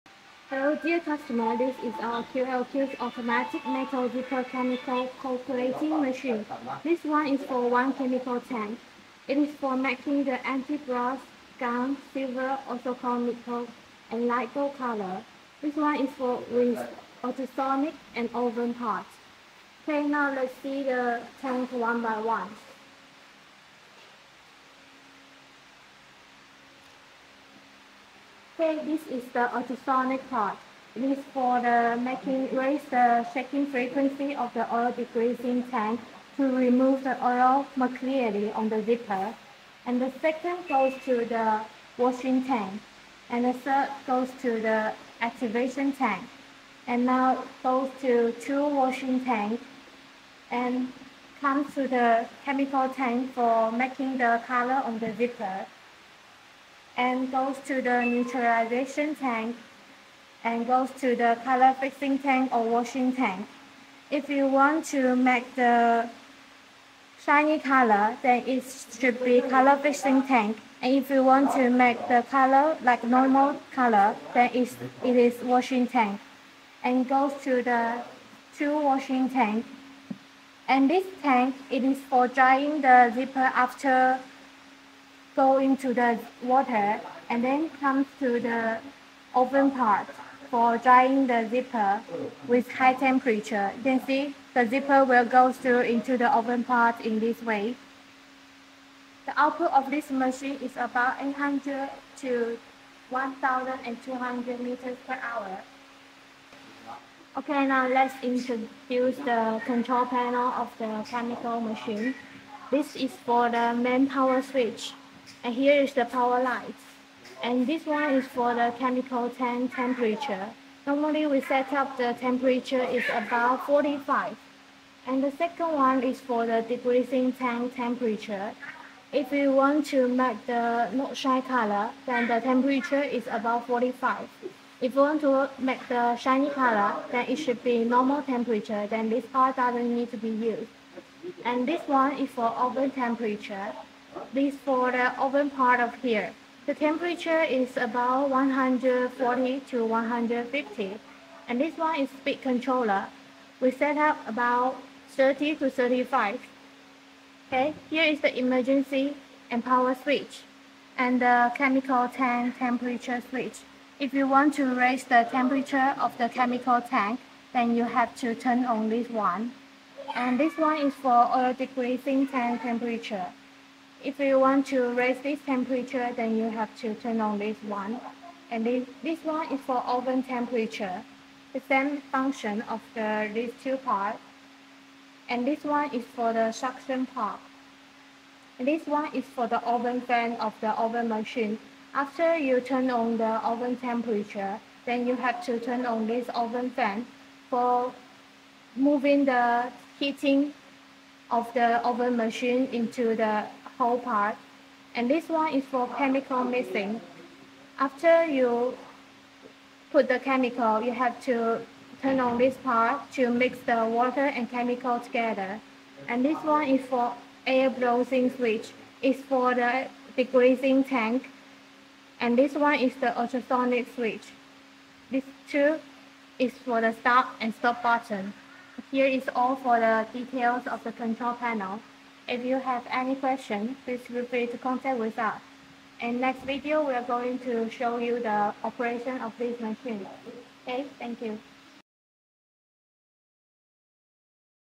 Automatic Metal Zipper Plating Machine with Oven and Ultrasonic Part platingmachine metal zipper
Elevate Your Metal Zipper with QLQ Plating Machines!
Model 2: Automatic Metal Zipper Plating Machine (one tank model, with oven and ultrasonic part)
Output: 800-1200 m/h